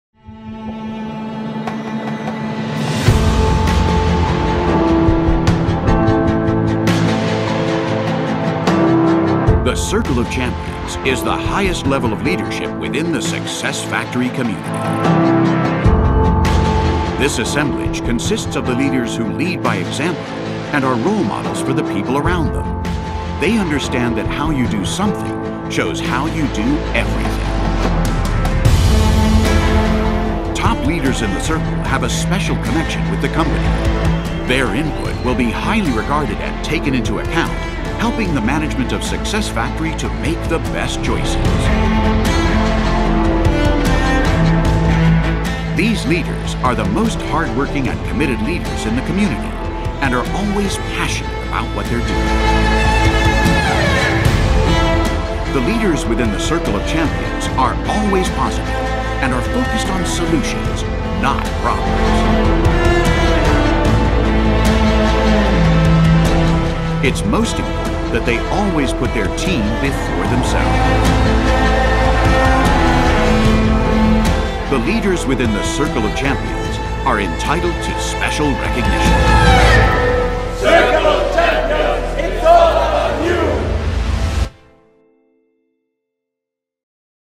Nuestro primer Círculo de Campeones
Un día se atrevieron a lograr sus sueños, buscando la excelencia en cada paso que daban. ¡Son parte de los que dijeron SI a la felicidad y a superar nuevos retos! Detrás de este reconocimiento hay esfuerzo, compromiso, visión, entrega... ¡Conoce a los primeros miembros del Círculo de Campeones! Un grupo de líderes realmente comprometidos con la visión y la misión de Success Factory. Un grupo en el que tú también puedes tener un lugar.